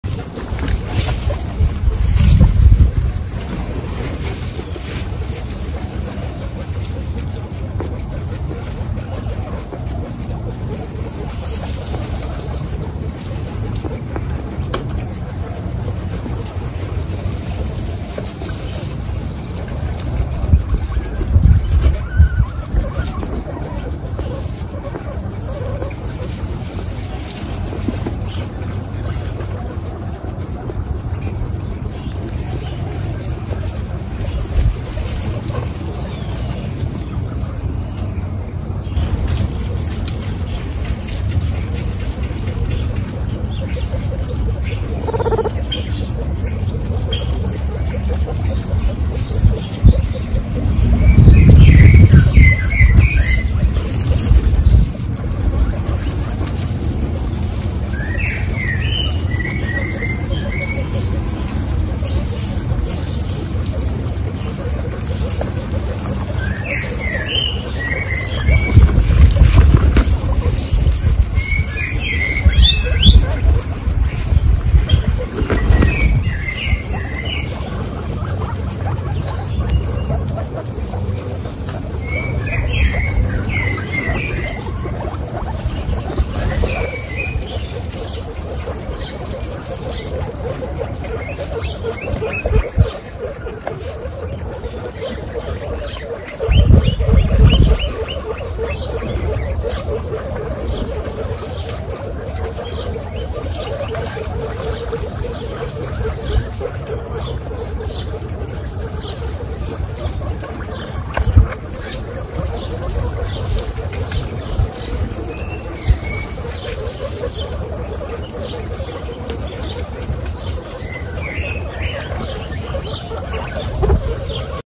Guinea pigs carrots Mukbang
Hi, I have 26 female Guinea pigs.
Broccoli, Brownie, Butter, Cappuccino, Choco, Coco, Coconut, Cookie, Cheese, Dumpling, Kiwi, Latte, Lion, Mango, Marshmallow, Mochi, Mozzarella, Muffin, Oreo, Pancake, Peach, Peanut, Potato, Puppy, Tofu, and Walnut.